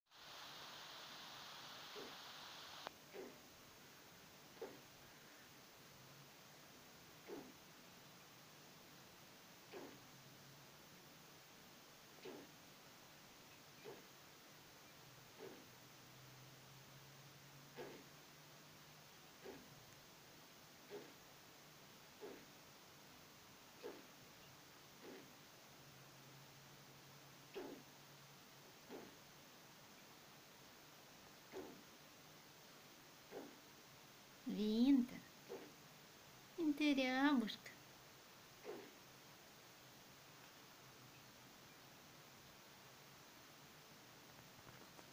Голубка Винтер-Рябушка говорит "Гу!"
Клюнутая вороной маленькая голубка Винтер-Рябушка отрастила хвост и очень уже хочет на улицу. Как её выпустить - не представляю 😪
Лапка поправилась сама по себе: за это я больше всего переживала потому, что было похоже на вывих сустава стопы. Но все хорошо, хотя утолщение осталось.